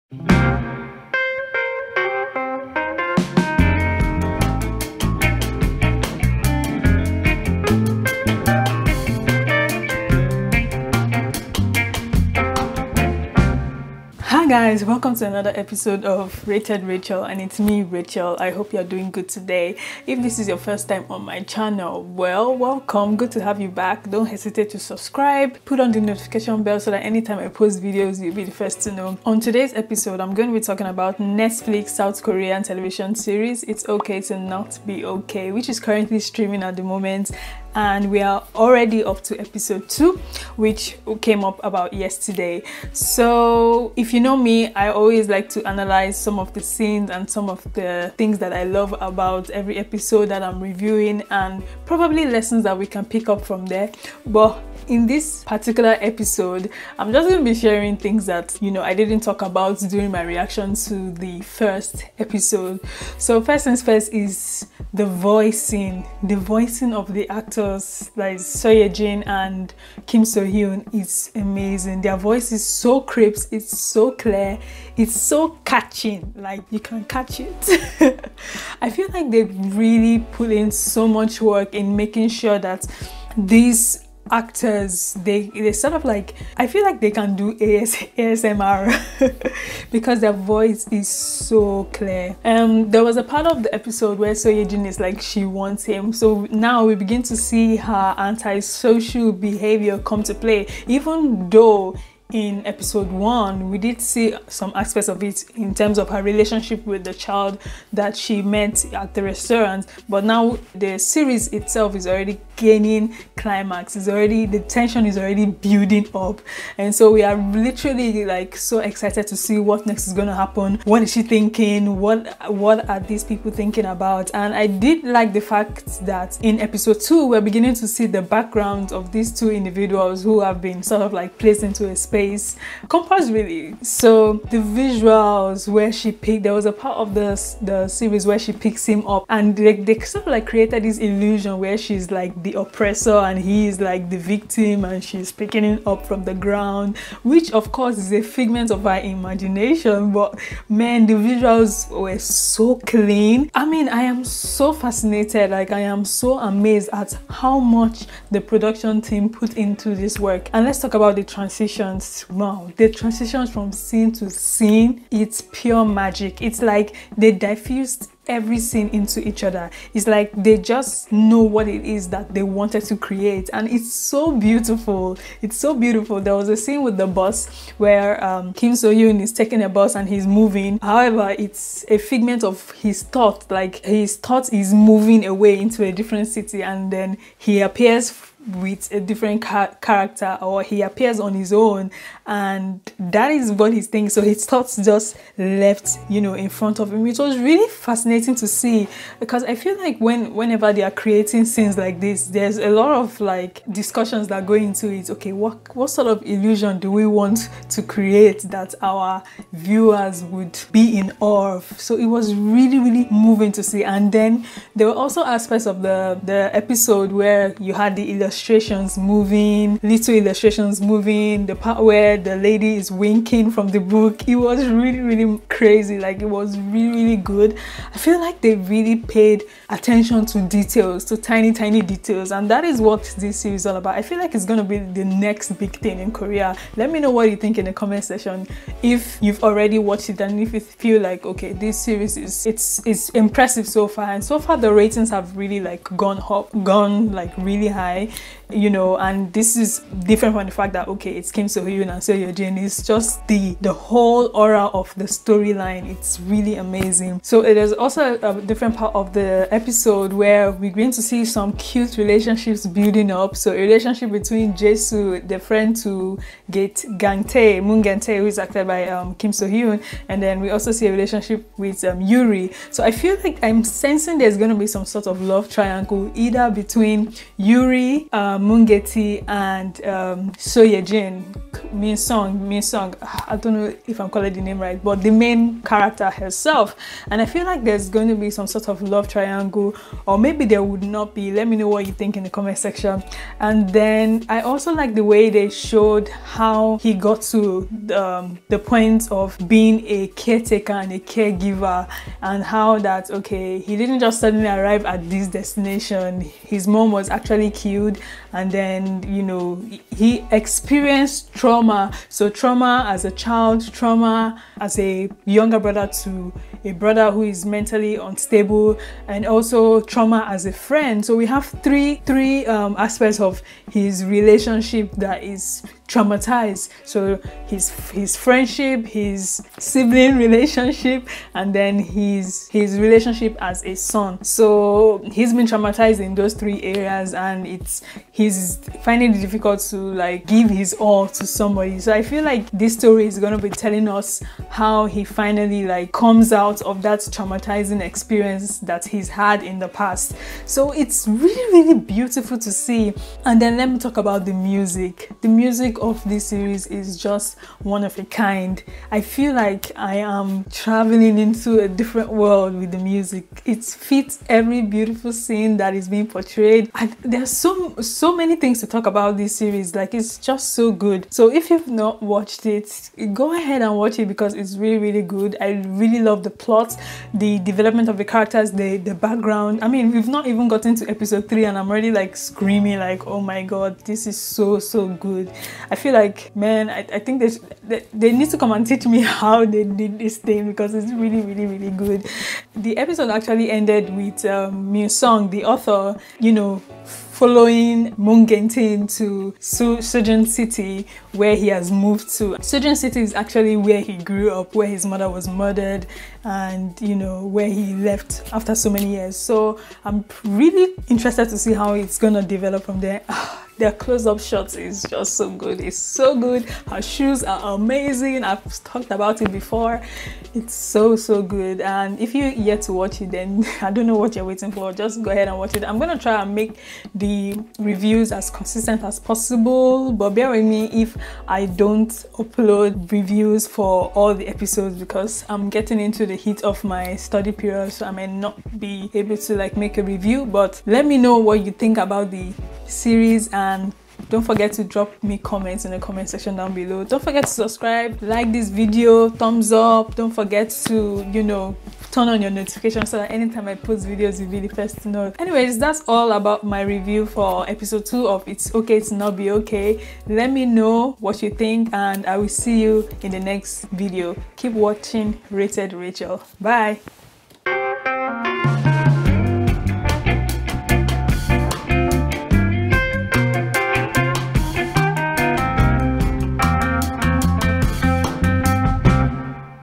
Netflix's It's OKAY TO NOT BE OKAY EPISODE 2 REVIEW - PURE MAGIC!!! | Rated Rachael
NETFLIX IT'S OKAY TO NOT BE OKAY EPISODE 2 REVIEW

Hey guys! Apologies about the late upload of Episode 2 review. These series is pure MAGIC!! Let me know what you think about the show.

BUILD YOUR OWN UNIQUE LINK (USING LYNX IN BIO)